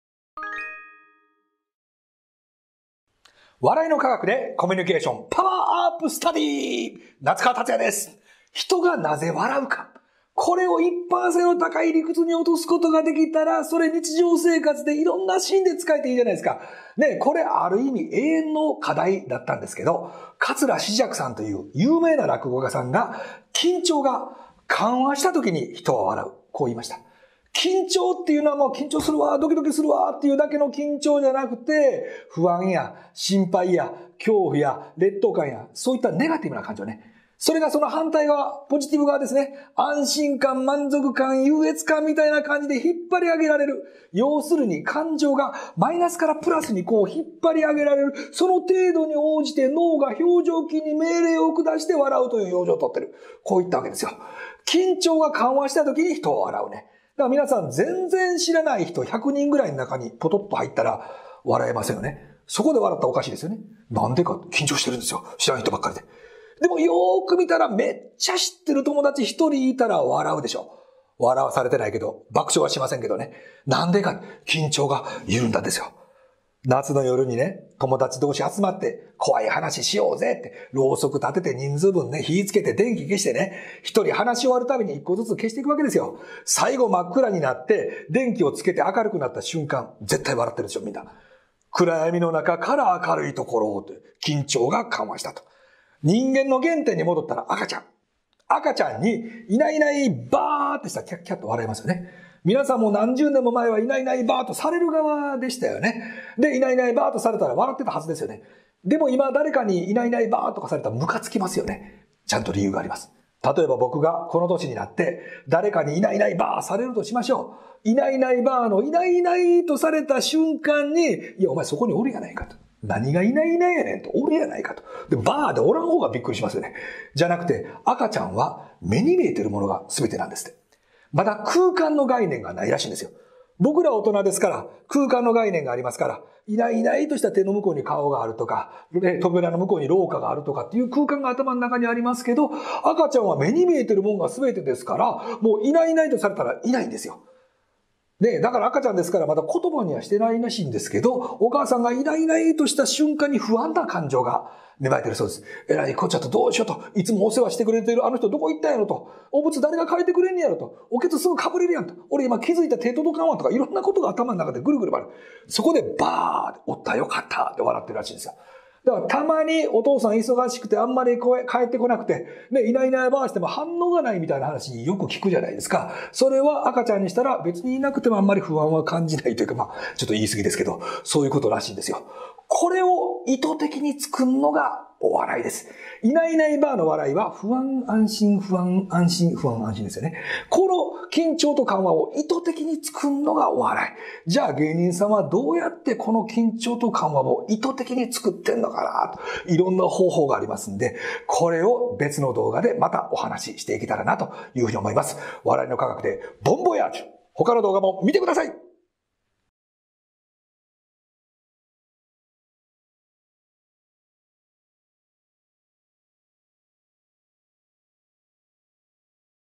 人はなぜ笑う？緊張と緩和「笑いの科学！ コミュニケーション パワーアップスタディ」夏川立也
「笑いの科学！　コミュニケーション　パワーアップスタディ」

お笑い芸人の「場の空気づくり」と「相手から引き出す」能力、それに「話す」技術！ これら笑いの法則を紐解き、誰とでも気持ちを伝えられる話し方ができるようになるノウハウ！

元祖京大卒芸人・夏川立也氏が、お笑い芸人が使っている笑いのテクニックを分解し、誰でも日常のコミュニケーションに使えるように法則に落とし込んで解説。話が苦手な人、なぜか思っていることが伝えられない人、職場やその場の雰囲気をよくしたい人、ぜひこのテクニックをとりいれてコミュニケーション力をパワーアップ！

いわゆる「お笑い」の笑いとは別に思わず笑みがこぼれることってありますよね。心配していたことがなくなった時、ほっとして無意識に笑顔になります。このようなテクニックを自然に使えたら、かっこいいですね～。

「コミュニケーション パワーアップスタディ」商品版も発売！詳細・購入はこちら